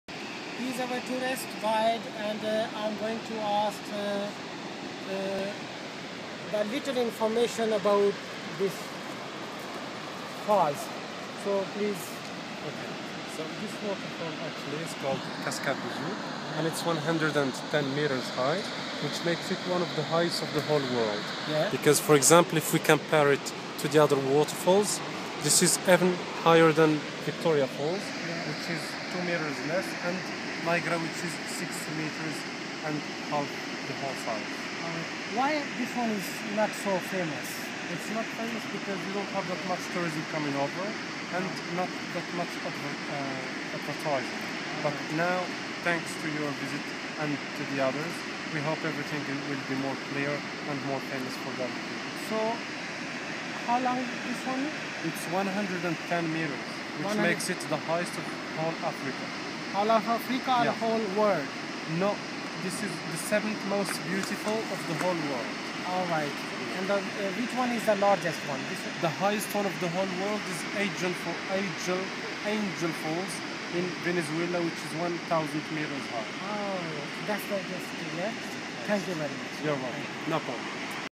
Ouzoud Waterfalls
Ouzoud Waterfalls (French: Cascades d'Ouzoud) (110 m high) are located in the Grand Atlas village of Tanaghmeilt, in the province of Azilal, 150 km north-east of Marrakech, in Morocco.

Ouzoud means "the act of grinding grain" in Berber, this theory is confirmed by the frequent mills in the region.[1]

The bottom of the falls is accessible through a shaded path of olive trees. At the summit of the falls, there exist a dozen of old small mills that are still in use. One can also follow a narrow and difficult track leading to the road of Beni Mellal while descending the gorges from the "wadi el-Abid" by a canyon sometimes which one does not distinguish the bottom with nearly 600 metres.

It is the most visited site of the region. In the vicinity, Green valleys, mills, orchards and a superb circuit of the gorges of the El Abid River (in Arabic, "Slaves' River" ), are found. Many local and national associations lead projects to protect and preserve the site.